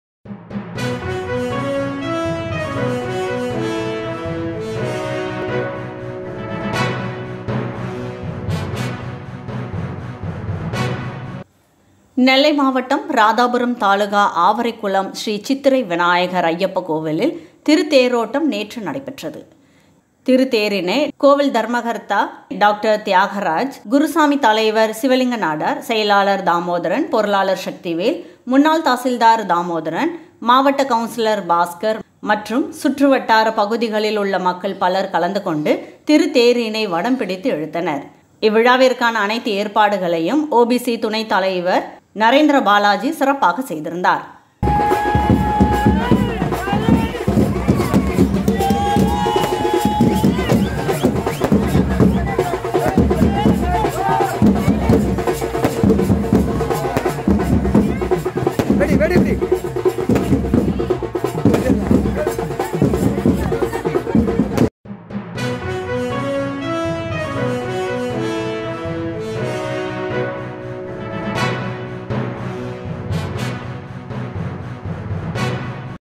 ராதாபுரம் தாலுகா ஆவரைக்குளம் ஸ்ரீ சித்திரை விநாயகர் ஐயப்பன் கோவிலில் தேரோட்டம் நடைபெற்றது |mayuri tv
ராதாபுரம் தாலுகா ஆவரைக்குளம் ஸ்ரீ சித்திரை விநாயகர் ஐயப்பன் கோவிலில் தேரோட்டம் நடைபெற்றது